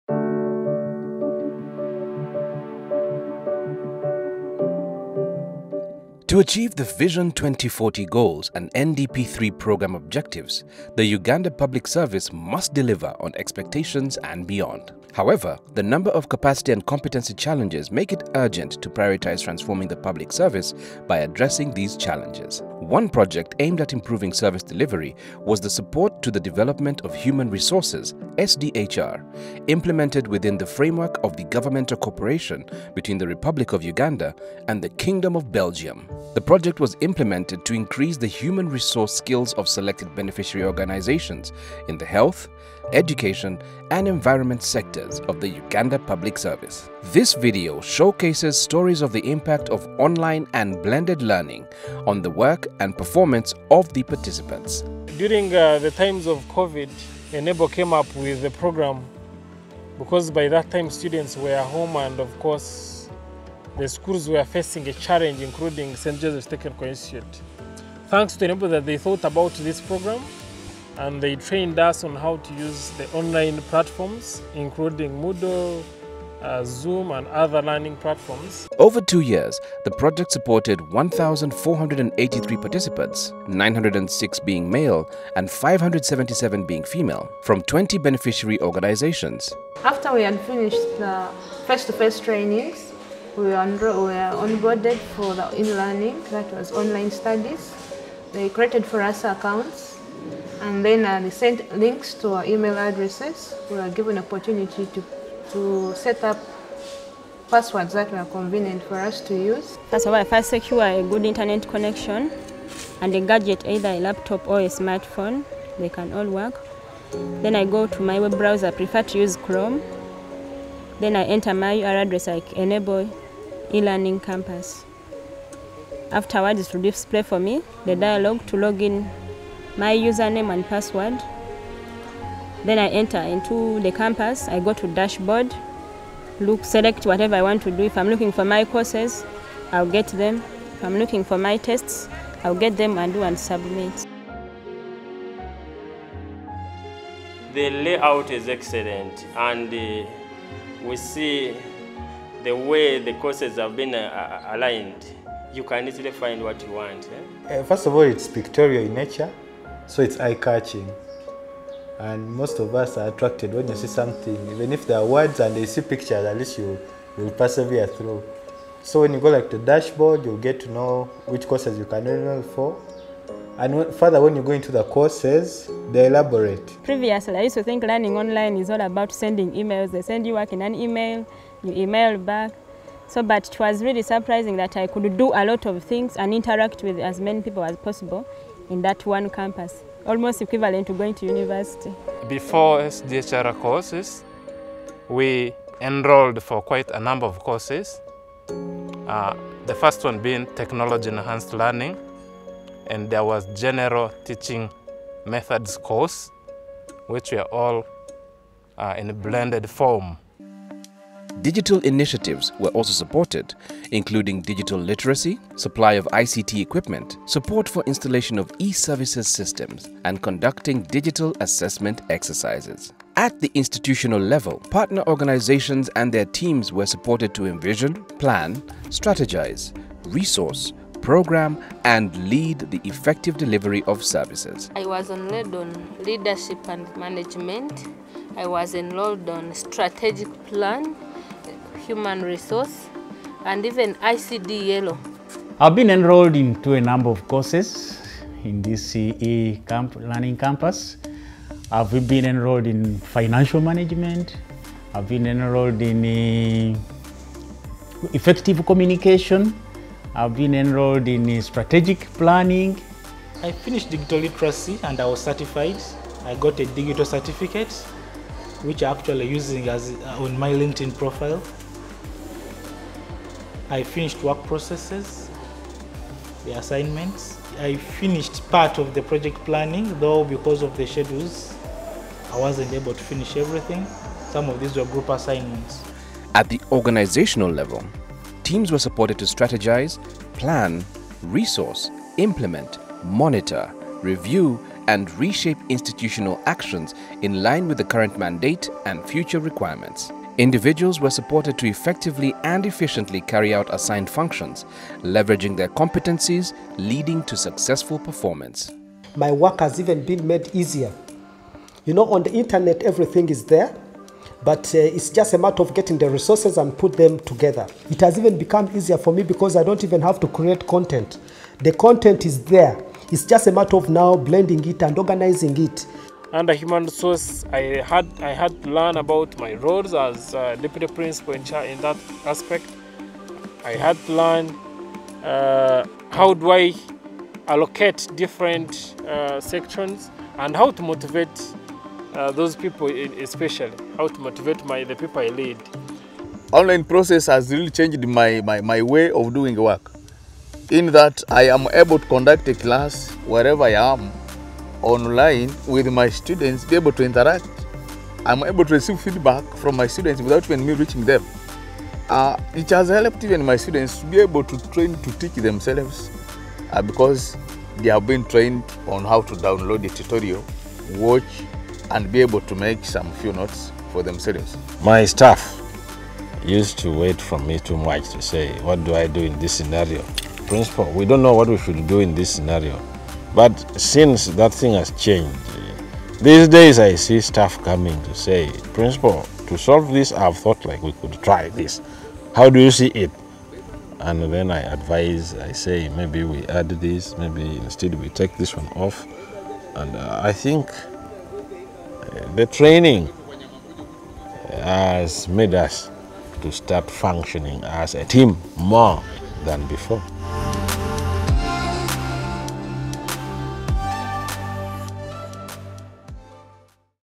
Support to the Development of Human Resources: eLearning success stories
Through the Support to the Development of Human Resources project, Enabel is supporting institutions to improve service delivery through online and blended learning.